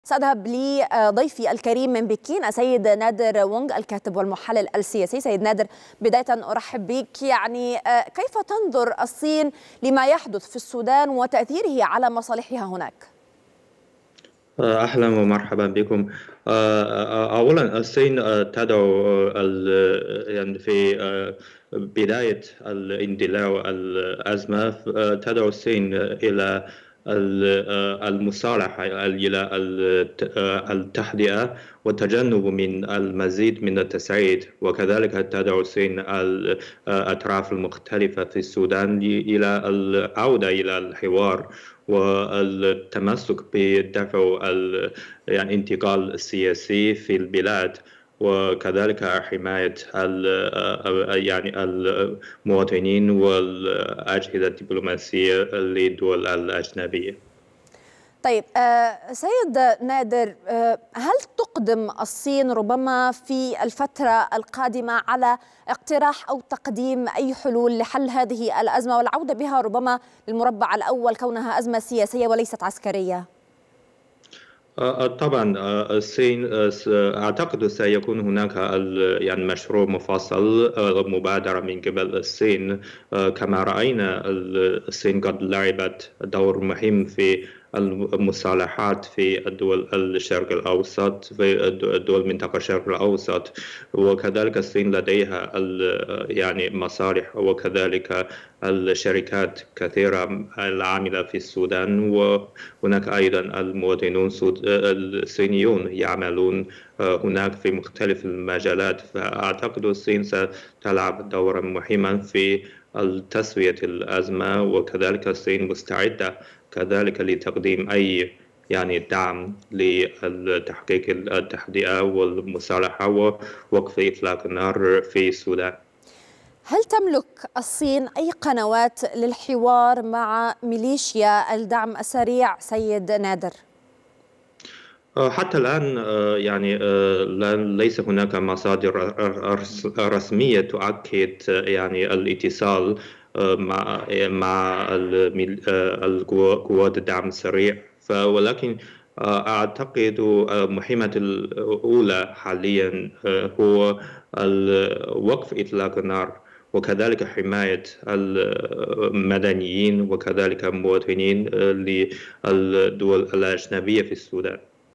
كيف تنظر الصين إلى الأزمة السودانية؟.. محلل سياسي يُجيب
كيف تنظر الصين إلى الأزمة السودانية؟.. نادر رونج الكاتب والمحلل السياسي يُجيب

19/4/2023